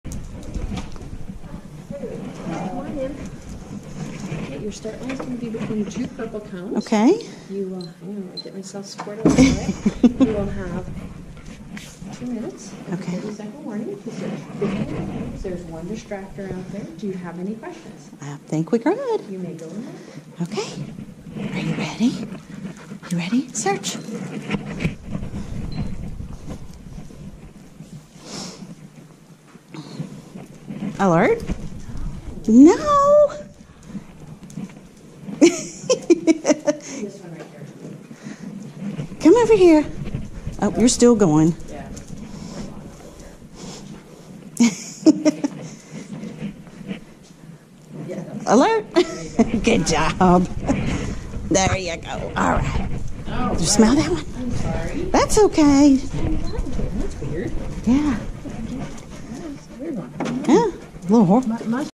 Scent Work Advanced Container Trial 2 NQ 2023 05 28
These are videos that i use to help me watch my dog so i can understand his body when he's alerting or getting ready to alert on scent or a rat.